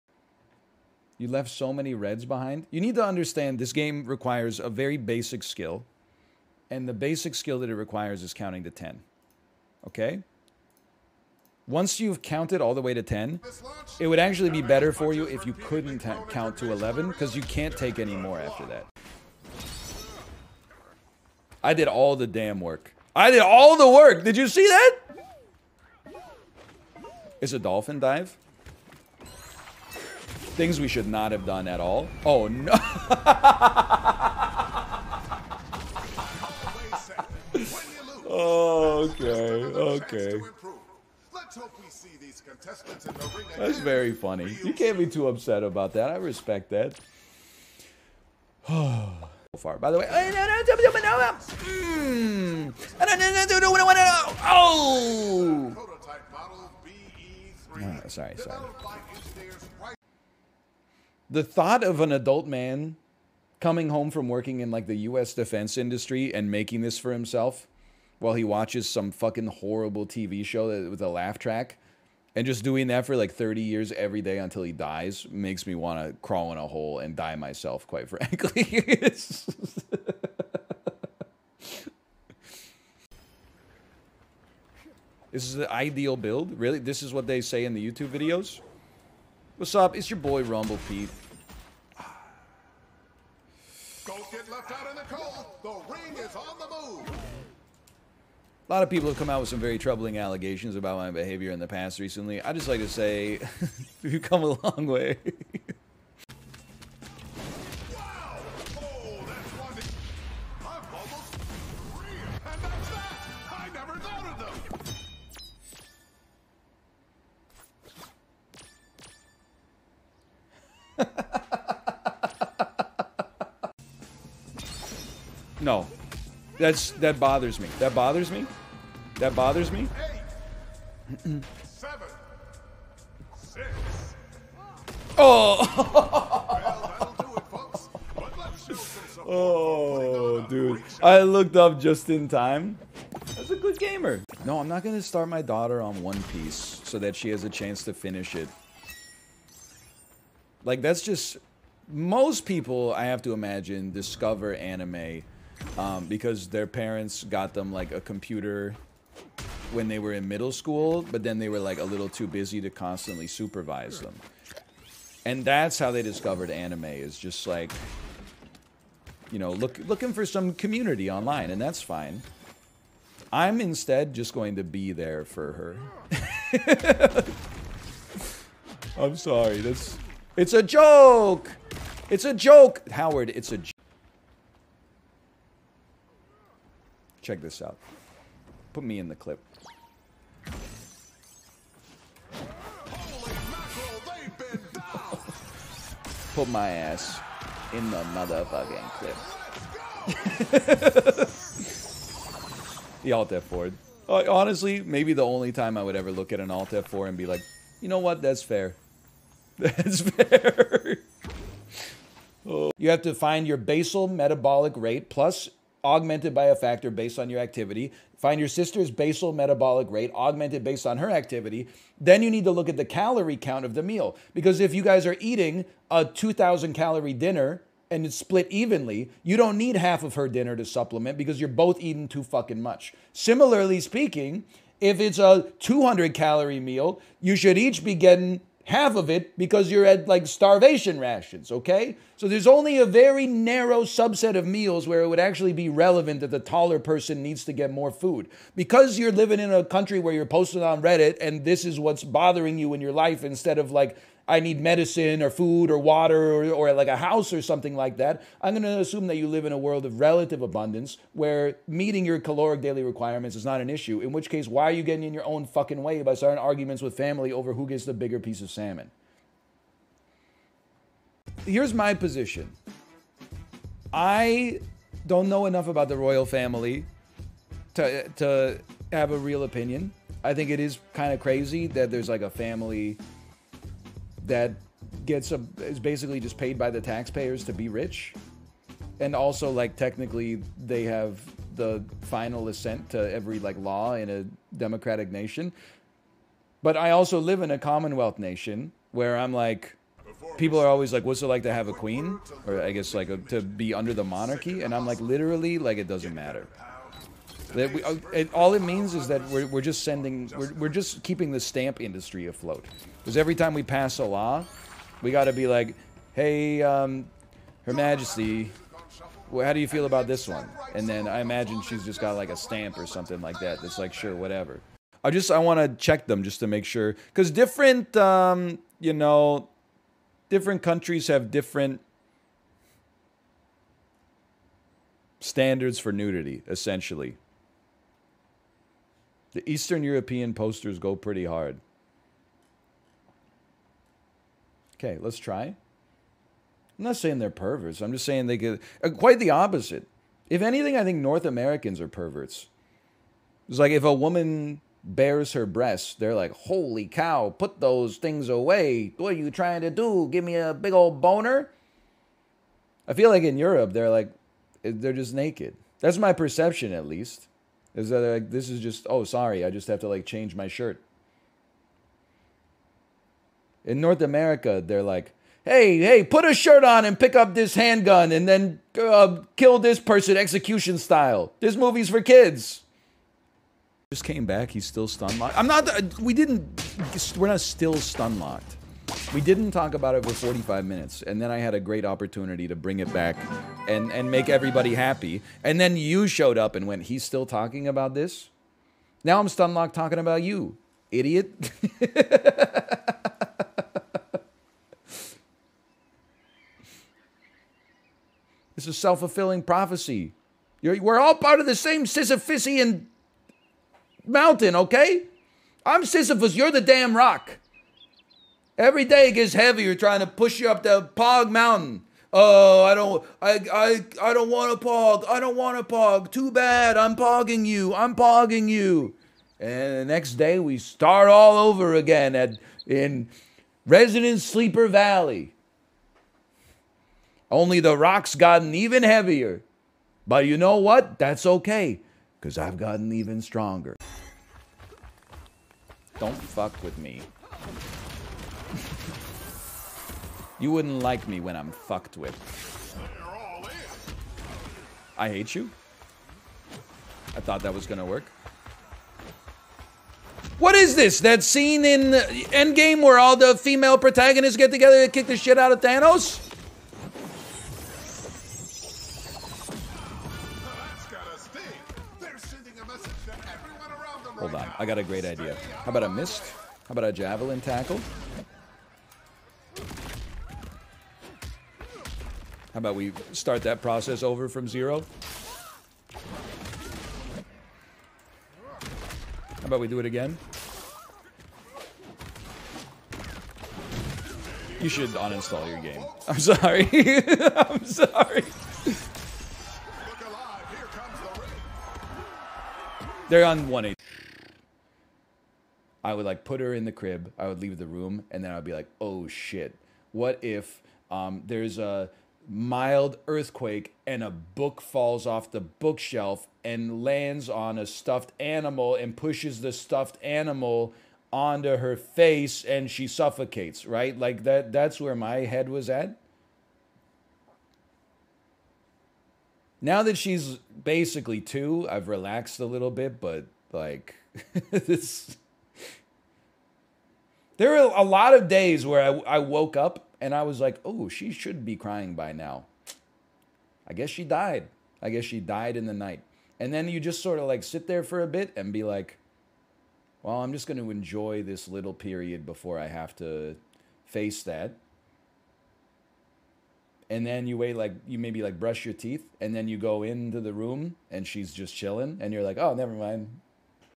he judge, he jury, he eggxecute | NL Twitch Highlights #116
0:00 - "friendly streamer helps u with ur math homework"
0:19 - "slash moment??"
0:49 - "peak NL " (by DoogieHowserSpankedMySon)
1:00 - "Reddit request" (by Mechsu)
1:23 - "rumble pete" (by m0ppu)
1:46 - "The win that broke NL's brain" (by TheRealScience86)
2:04 - "Don't Look Up (the NL rewrite)" (by 71mb3r)
2:24 - "nl elbow drops chat" d
3:04 - "NL marks player for deletion"
3:33 - "NL goes off" (by ViolenceOfDucks)
4:31 - "Northernlion on the Queen" (by AshLikeThat)
5:25 - "in north america they're like this.. but in europe they're like THIS" (by himurastewie)
6:24 - "NL’s Sisyphisean Mountain" (by tide_caesar)
7:24 - "C-C-C-COMBOOOOOOOO" (by g0ggy)
8:24 - "pyscho mode ryan" (by iDenahi)